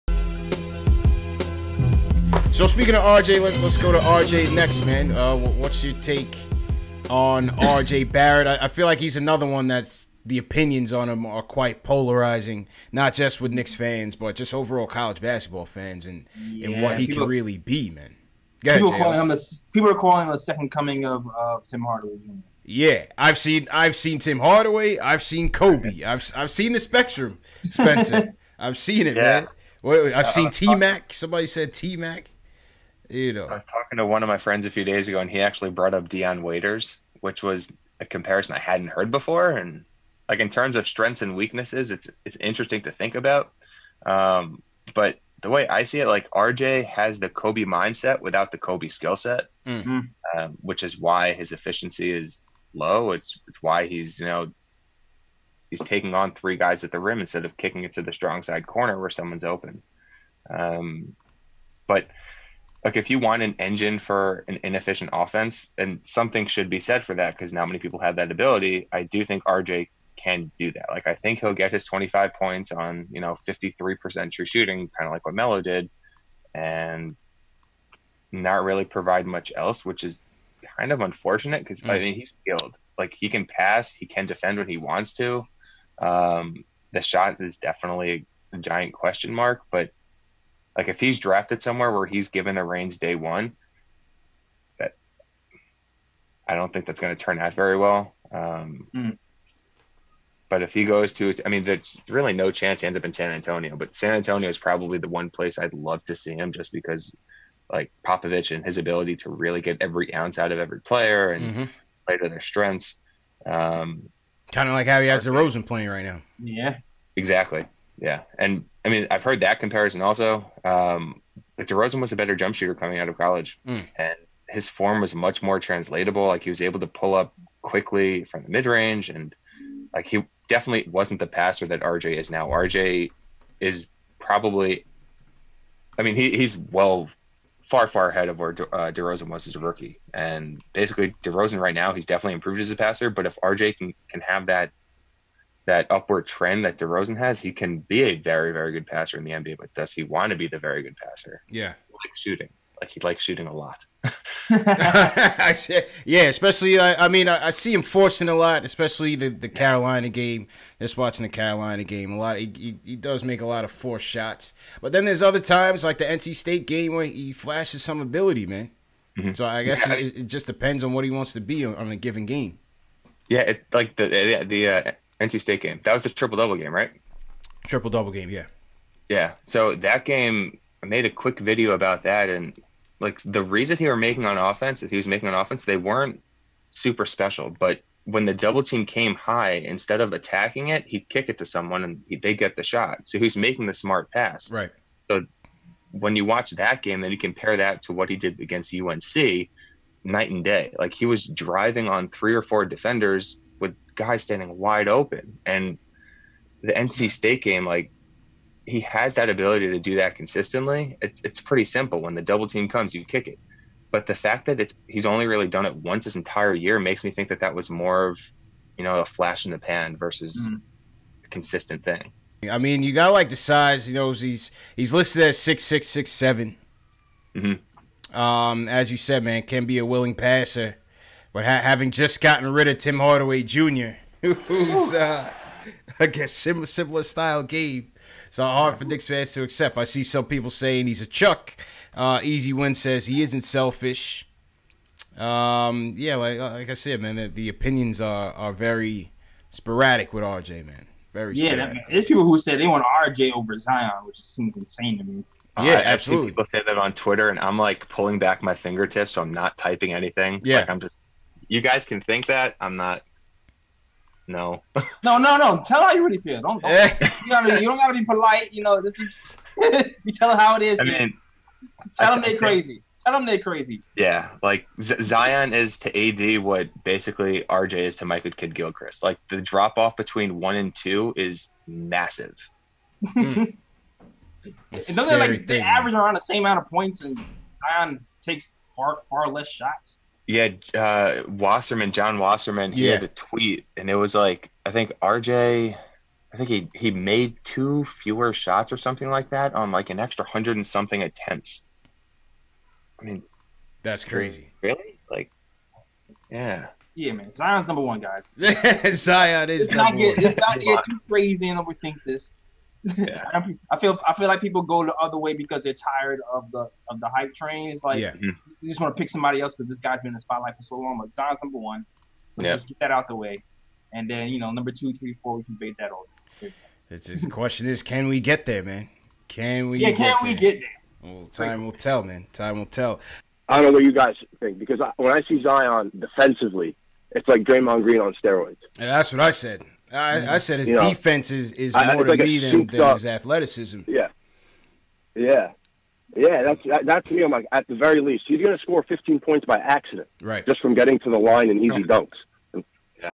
Everything You Need To Know About RJ Barrett| Strengths & Weaknesses| Knicks Draft Talk Ep. 2
RJ Barrett STRENGTHS & WEAKNESSES| NBA Draft 2019 Scouting Profile.  Can RJ Barrett become a complete player?  NBA Scout, Spencer from The Stepien joins CP and J. Ellis to give his scouting analysis of RJ Barrett.

🏀PODCAST LINKS! 🏀

2 ) 🔽Watch These Knicks Video Next!🔽